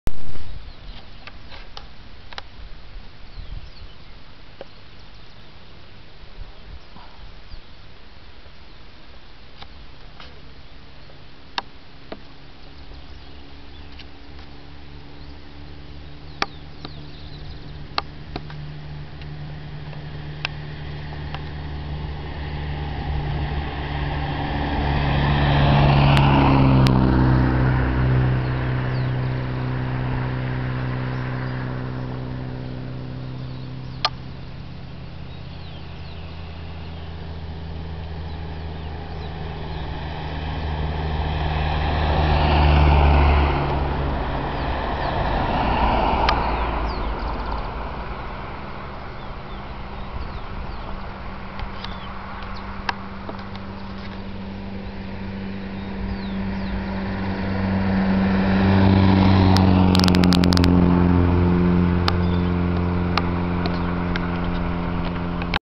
24 DE ENERO 2010: DIA DE TORMENTAS EN CATAMARCA (ARGENTINA) vid 3
Este video lo filme en Las juntas, a 40 km de la ciudad capital hacia el noroeste. Es un lugar humedo, con Bosque montano de Yungas, en el video se ven los Bosques de "Pino del cerro" y obviamente la formacion de mammatus, no muy desarrollados sobre la zona, recien habia terminado de llover. Habia tambien cumulus congestus y cumulus capilatus en el area...